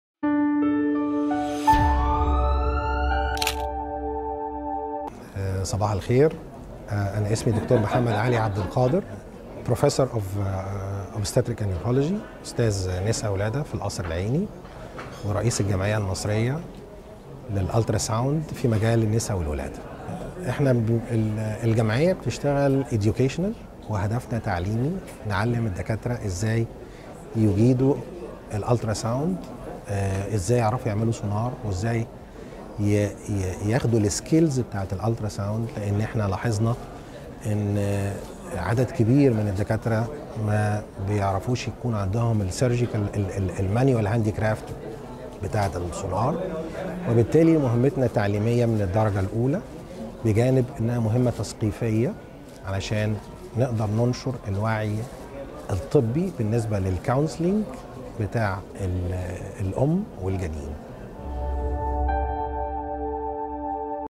الأستاذ الدكتور محمد علي يتحدث عن دور الجمعية في نشر الوعي الطبي في مجال السونار بين الأطباء
الأستاذ الدكتور محمد علي عبد القادر
أستاذ امراض النساء و التوليد بكلية طب القصر العيني و رئيس الجمعية المصرية للموجات فوق الصوتية في مجال النساء والولادة